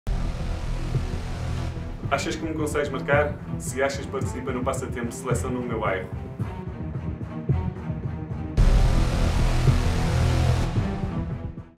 selecaonomeubairro - Cardinal
Queres a Seleção no teu bairro? Aceita o desafio do Cardinal, mostra-nos as tuas skills e as jogadas da tua equipa e habilita-te a jogar com os teus ídolos!